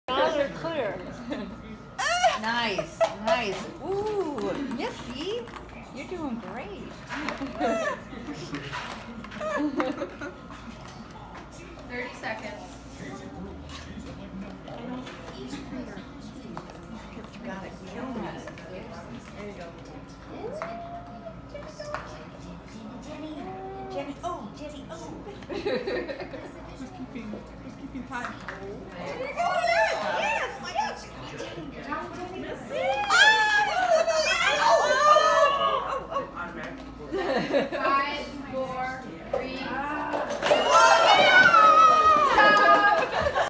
Jenny and Missy playing a Minute to Win It Game
Minocqua Aug 2018